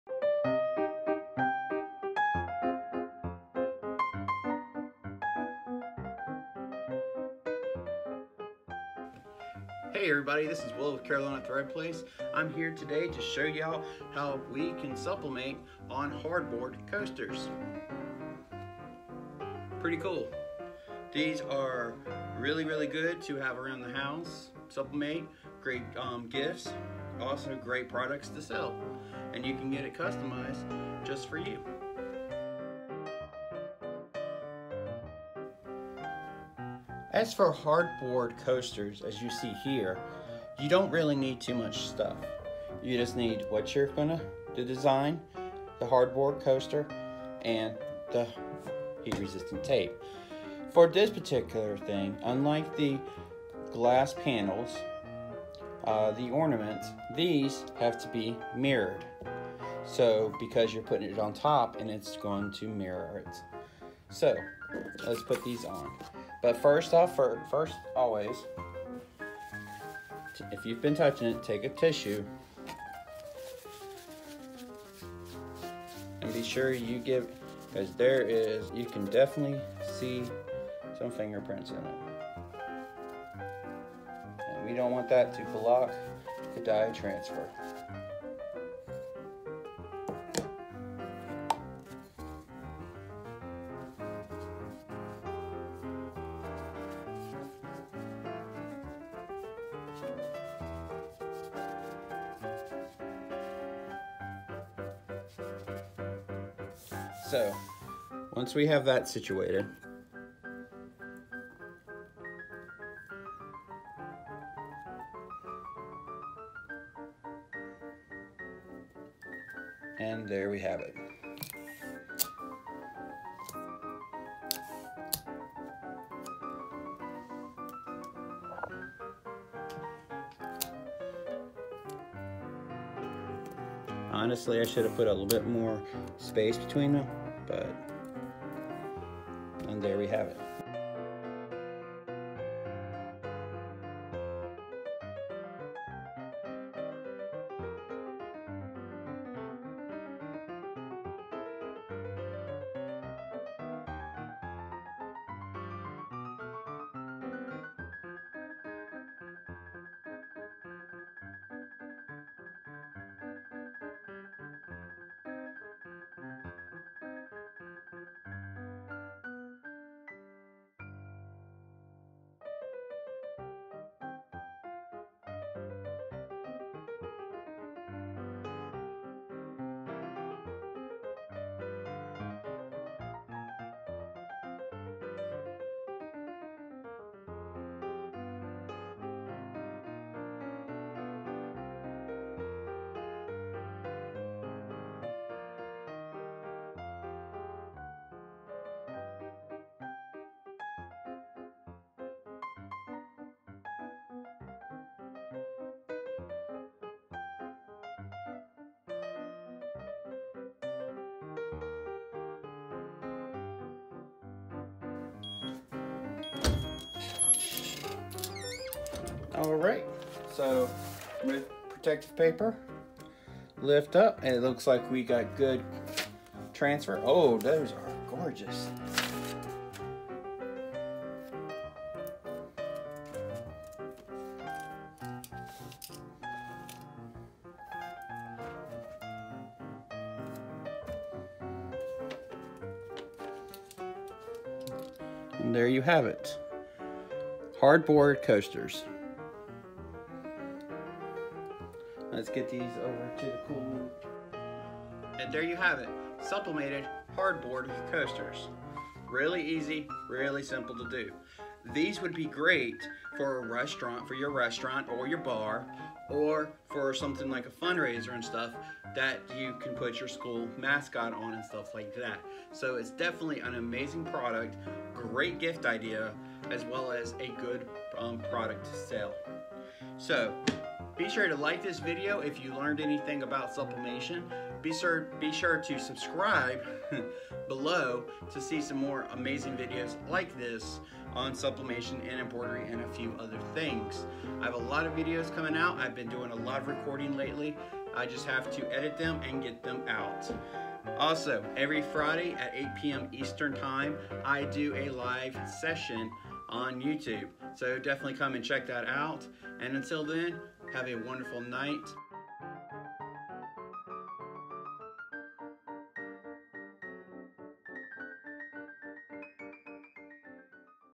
Hardboard Coasters | Carolina Thread Place
Hardboard Coasters

In the video today, I demonstrate how to sublimate on Hardboard Coasters.